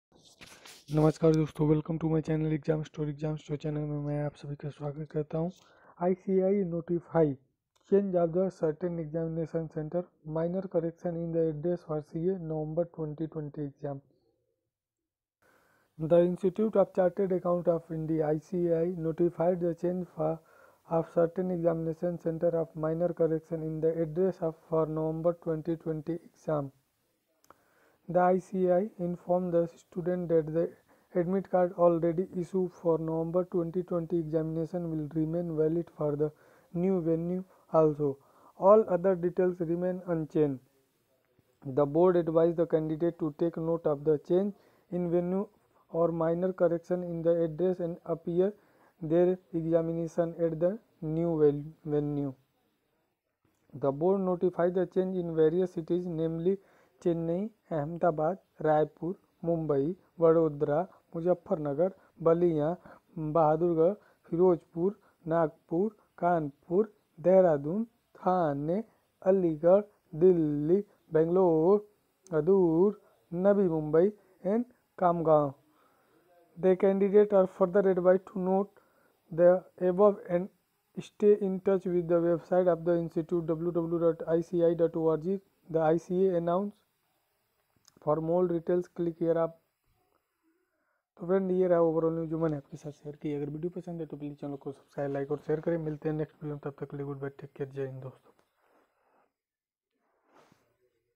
ICAI notifies Change of certain Examination Centre / minor correction in the address for CA Nov 2020
ICAI notifies Change of certain Examination Centre / minor correction in the address for CA November 2020 Exams